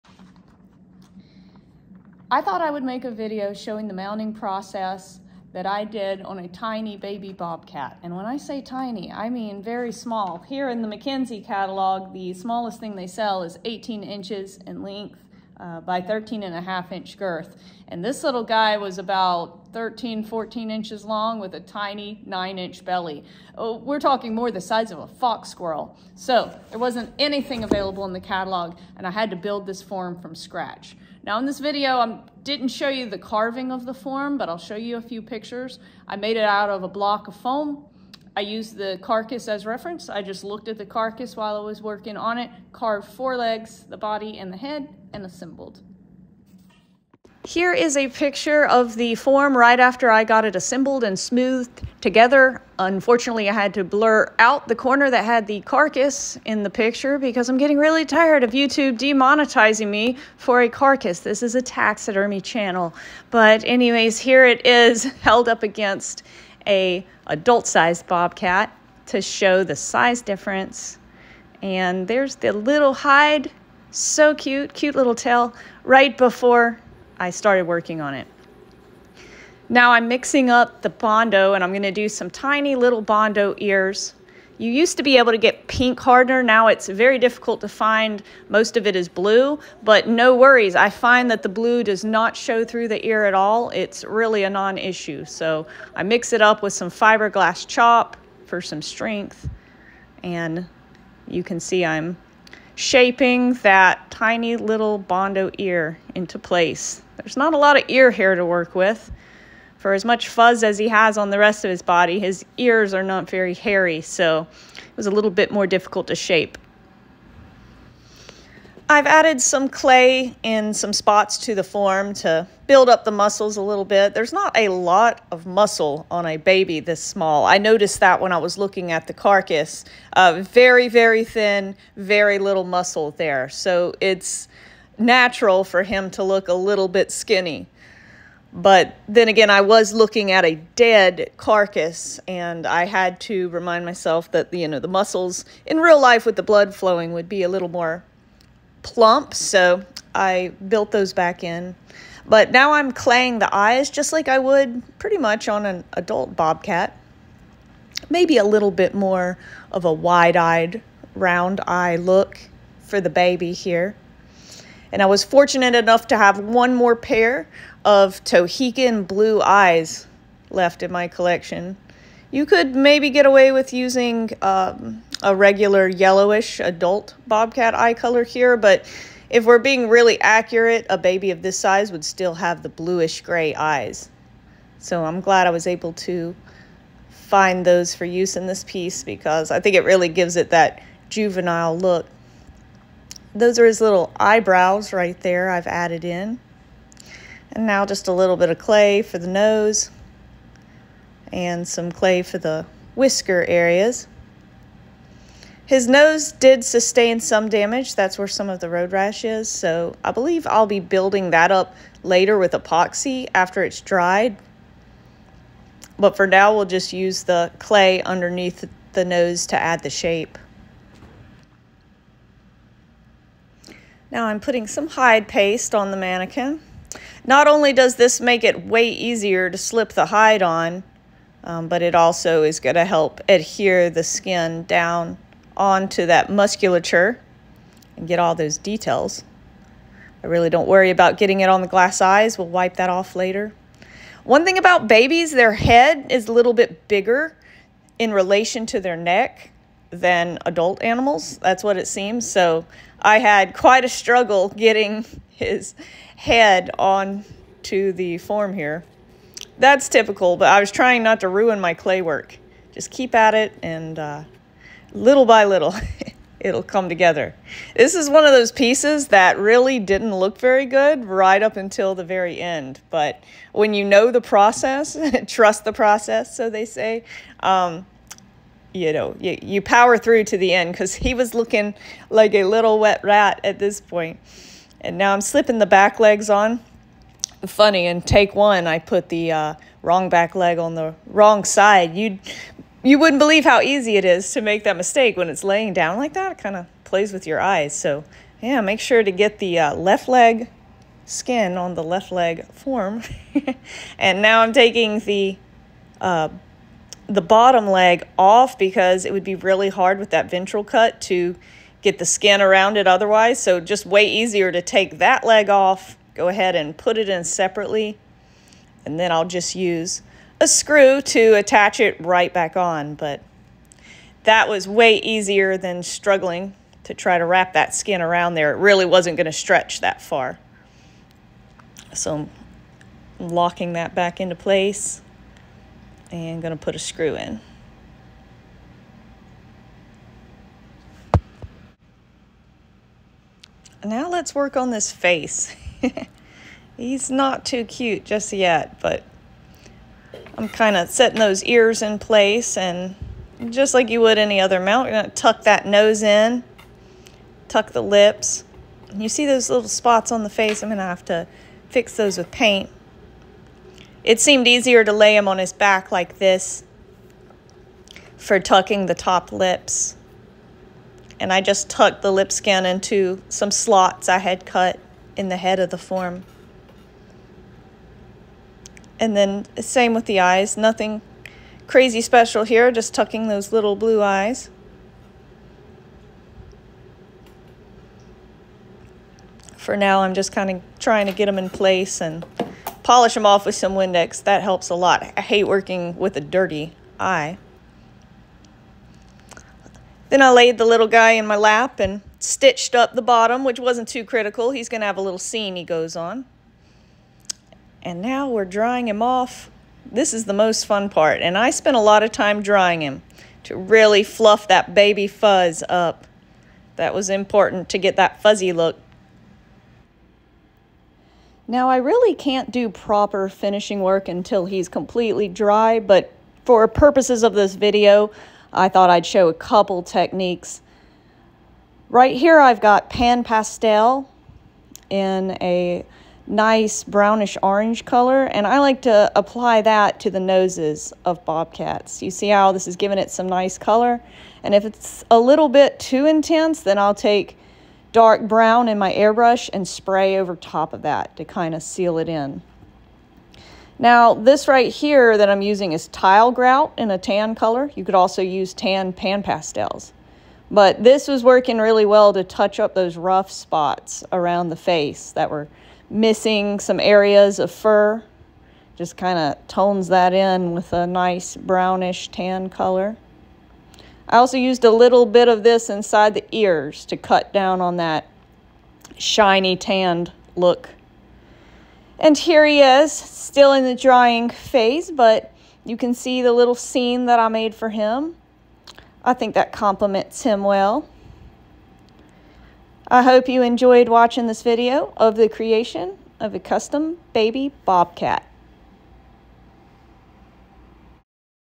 Baby bobcat taxidermy - assembly on a custom carved form!
In this video I assemble a very young bobcat on a hand carved form